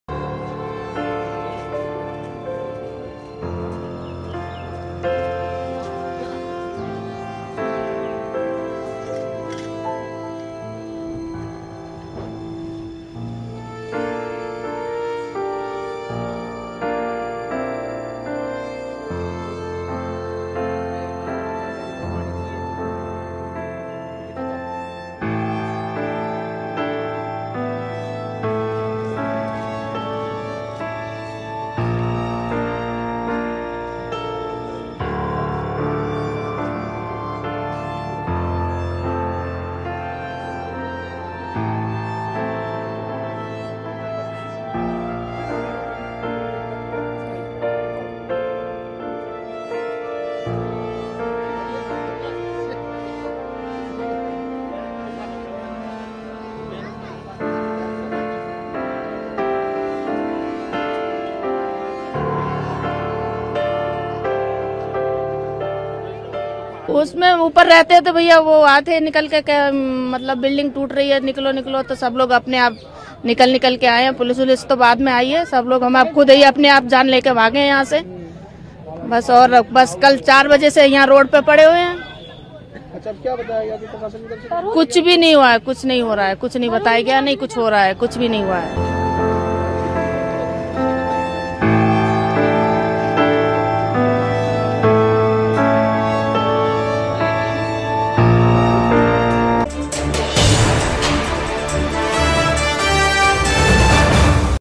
दिल्ली के मुनिरका में रातों रात झुकी 6 मंजिला इमारत, 100 से ज्यादा लोगों पर मंडराया जान का खतरा
दिल्ली से हैरान करने वाली खबर सामने आई है। यहां के के मुनिरका ( Munirka ) इलाके में रातों रात एक 6 मंजिला इमारत झुक गई। जिससे इलाके में अफरा-तफरी मच गई। आनन-फानन में पूरी बिल्डिंग को खाली करवाया गया।  बता दें कि बिल्डिंग करीब  45 किराएदार रहते थे। वहीं आसपास रहने वाले करीब  100 लोगों को भी वहां से हटाया गया। लेकिन उनके सामने सबसे बड़ी समस्या है कि अब वे कहा जाएंगे। यहां देखिए कैसे रातों रात झुकी 6 मंजिला इमारत।